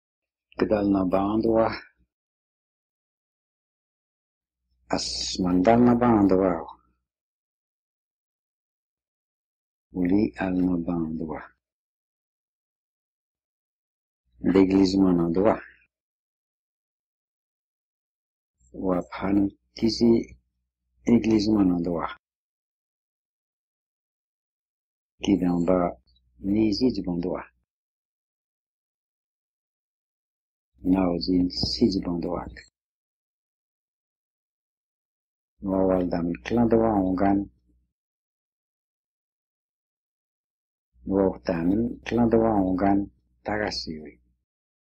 Speaking Abenaki
Essential phrases in Abenaki about speaking languages by Ambrose Obomsawin.